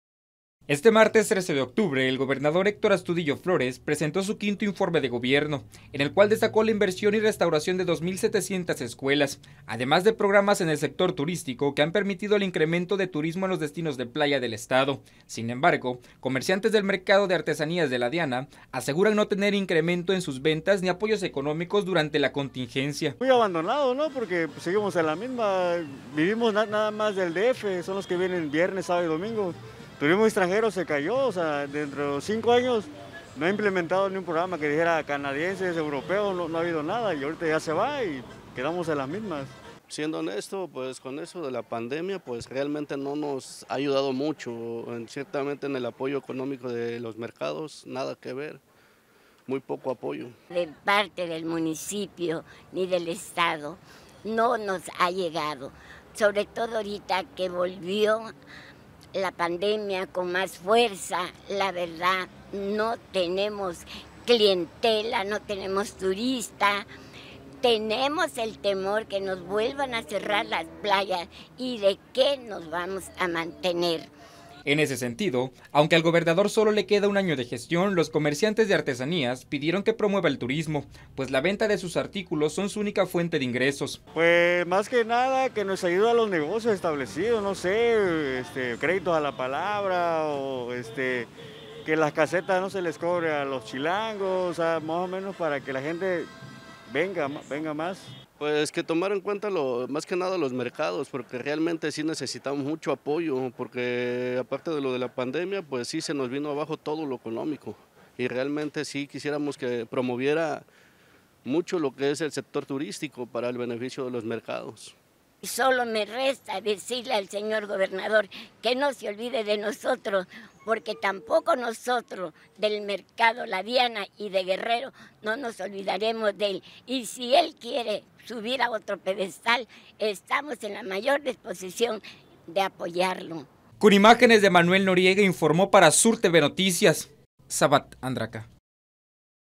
COMERCIANTES OPINAN DE LA GESTIÓN DEL GOBERNADOR
A 5 años de gobierno de Héctor Astudillo, comerciantes de artesanías de Acapulco aseguran no ser beneficiados por programas del gobierno estatal, urgen créditos para poder hacer frente a la crisis económica ocasionada por el COVID-19.